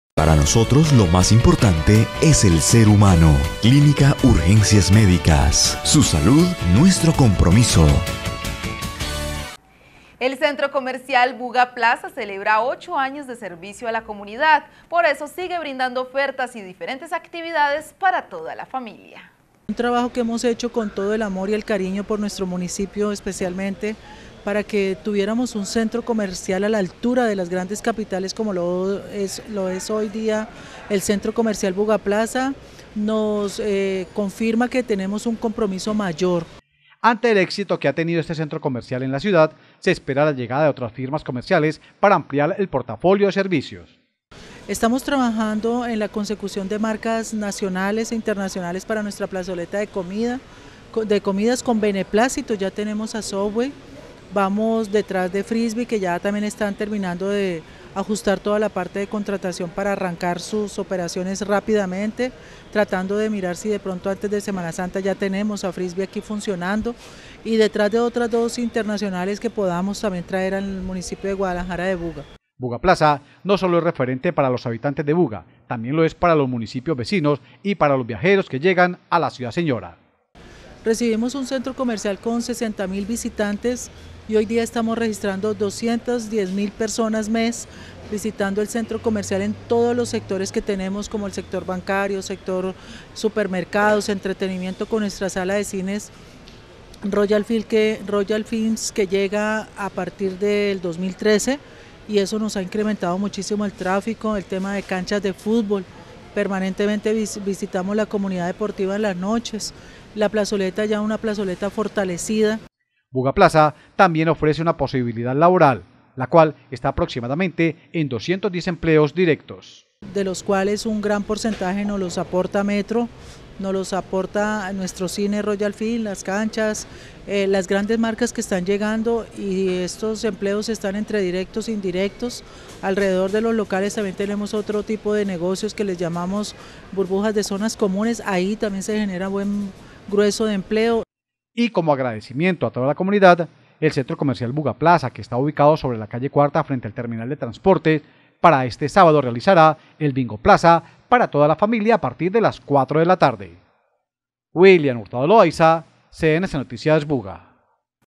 Centro Comercial Buga Plaza, 8 años de servicio a los centro vallecaucanos
El centro comercial Buga Plaza está celebrando 8 años de servicio a la comunidad, por eso sigue brindando ofertas y diferentes actividades para toda la familia.